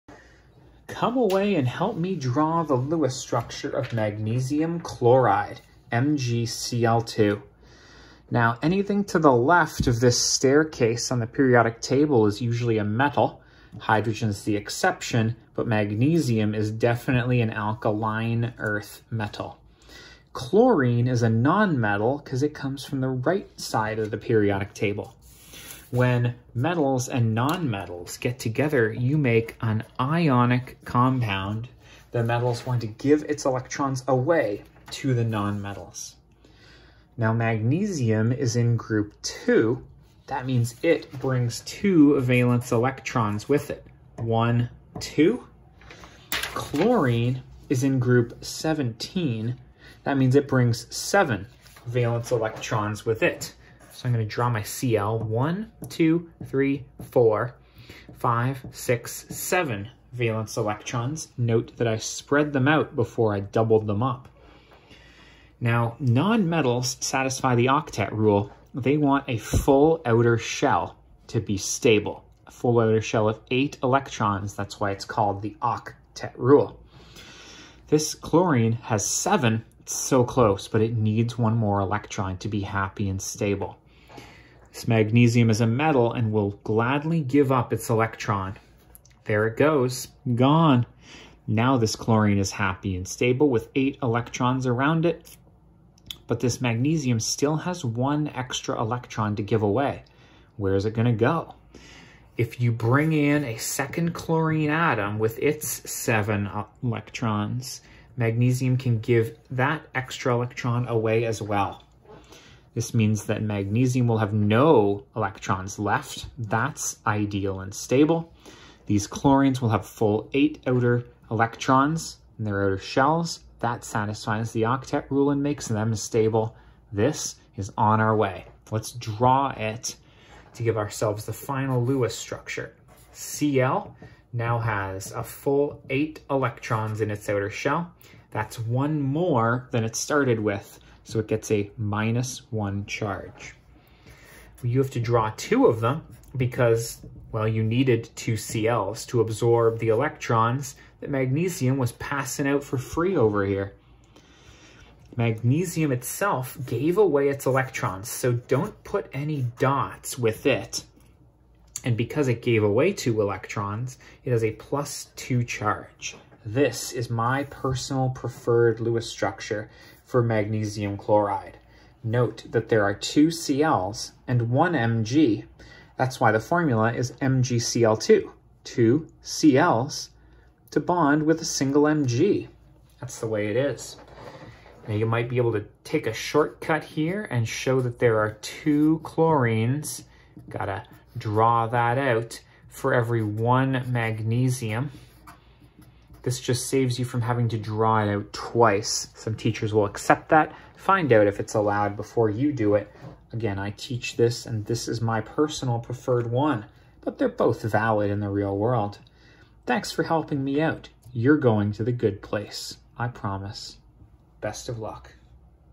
Draw the Lewis Structure of MgCl2 (magnesium chloride)
One magnesium atom loses two electrons, to become a +2 ion (cation).
Two chlorine atoms gain one electron each to become two -1 ions (anions).
These are held together with an ionic bond. Voila, magnesium chloride, MgCl2.